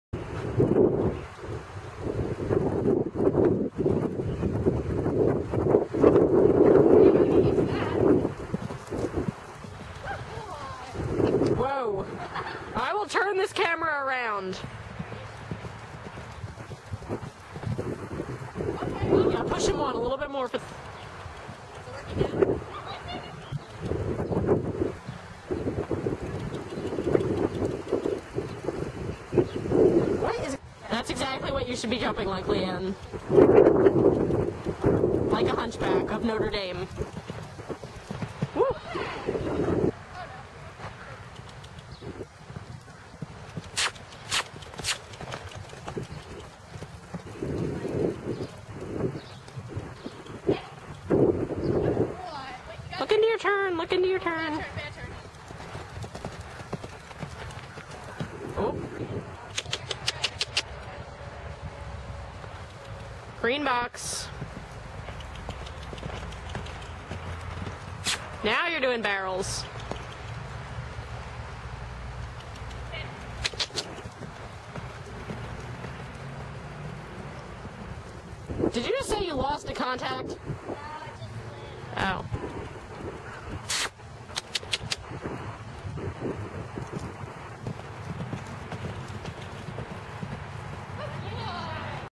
Lesson // 6.13.11 // Me jumping Lance.
Look everyone! It's Lance! You haven't seen me on him in a while have you now!? Haha, I missed riding him so I decided to ride him for my lesson yesterday! AH so much fun jumping him! He was really good! Had to get used to his cantering/jumping again but yeah loved it! Enjoy!